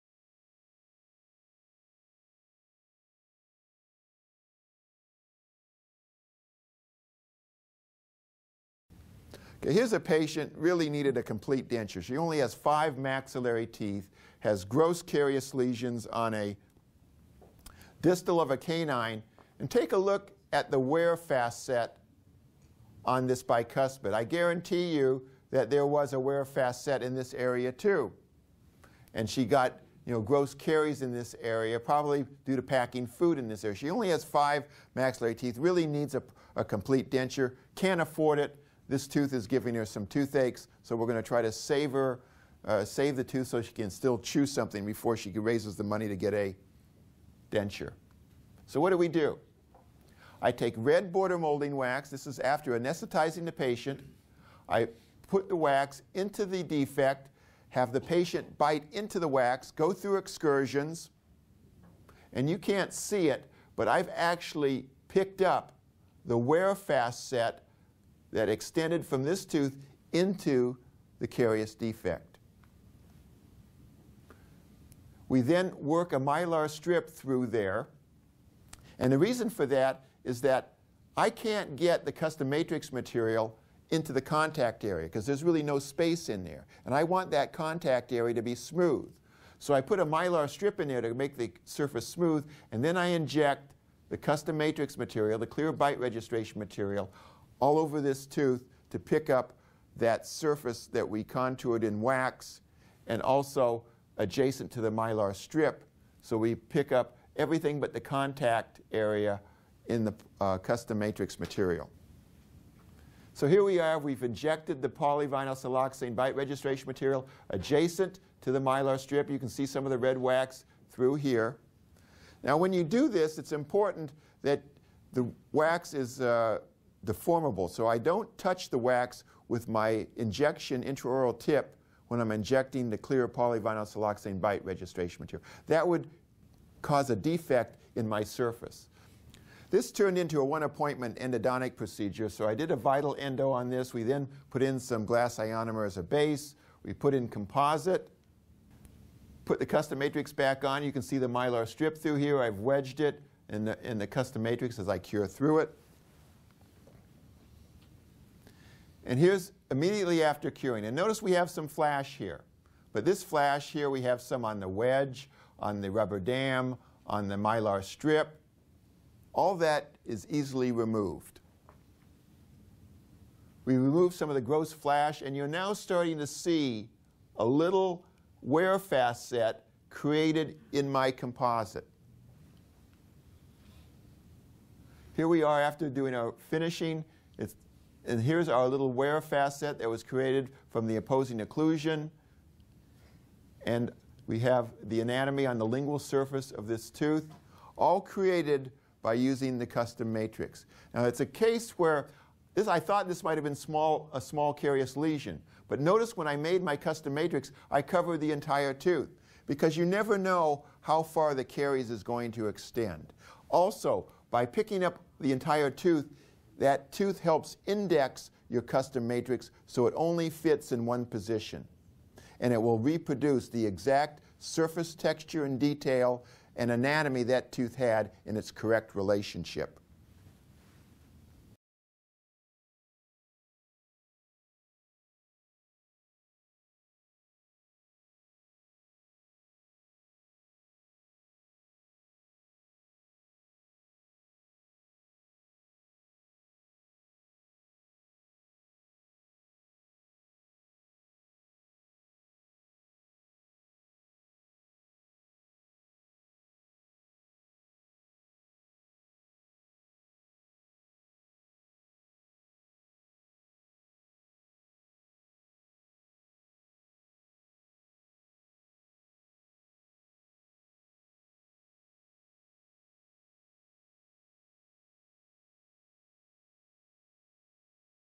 Large Class III
This clinical case utilizes the Clear Custom Matrix Technique to place a large light-cured Class IIi composite restoration in a maxillary canine. This clinical case was part of a continuing education lecture presented and video taped at Loma Linda University School of Dentistry. It is recommended that you watch my Maxillary 1st Molar (#3) MO Light-Cured Composite video first on YouTube since it gives more detail about the Clear Custom Matrix Technique. If you know the technique yo will better understand how you can save 5 to 10 minutes of chair-time on each composite you place.